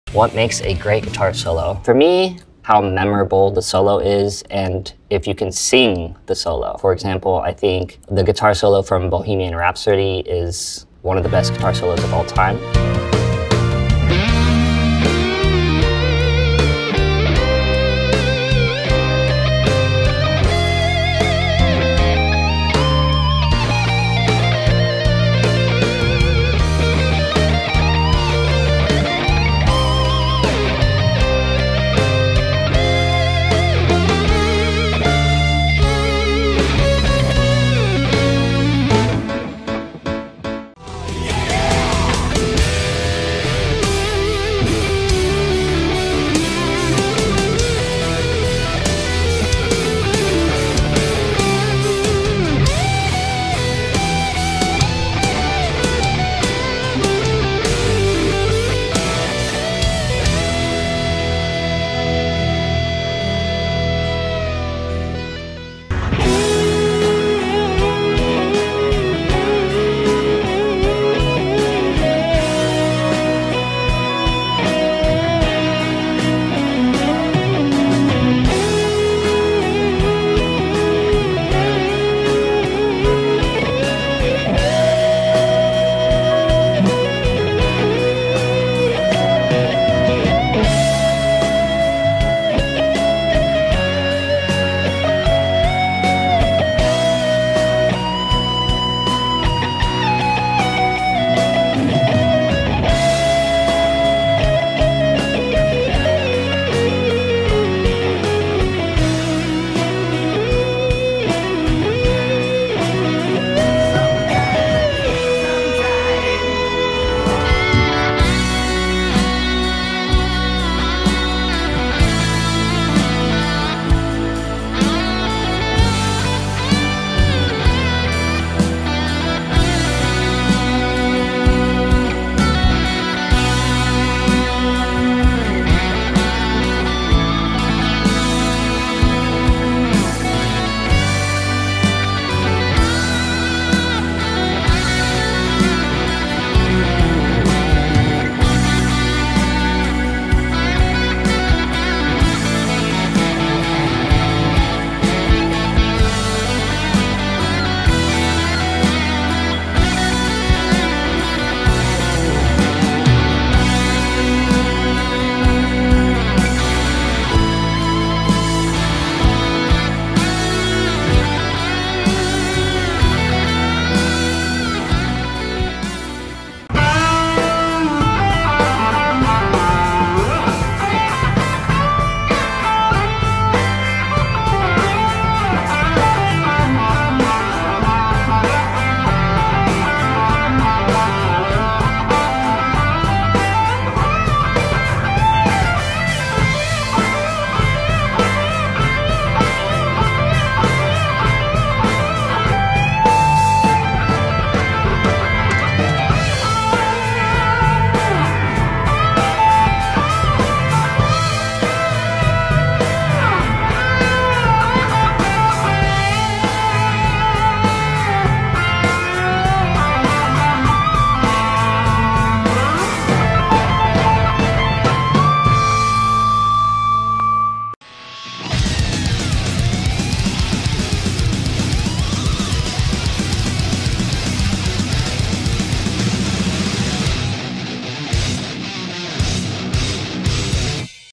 Top 5 Guitar Solo 🎸 Rock Edition + Tabs
The Greatest Guitar Solos of All Time Cover by Beautiful Girls

00:00 Intro
00:13 Federica Golisano
00:42 Fernanda Valderrama
01:06 Chloé
01:57 Parisa Delfani
02:56 Ocean Crosby